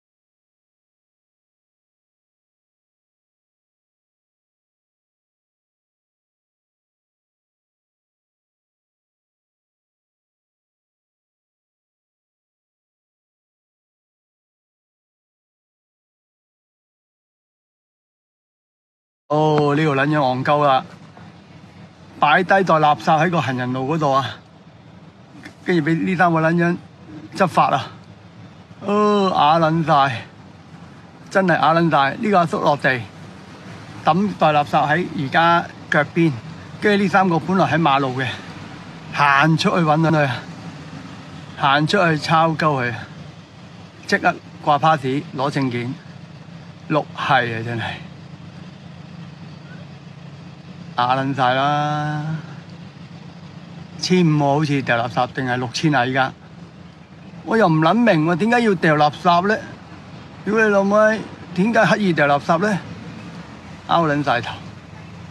車CAM直擊 - 世事就係咁蹺🤣亂拋垃圾撞正便衣
轉自: Wtsapp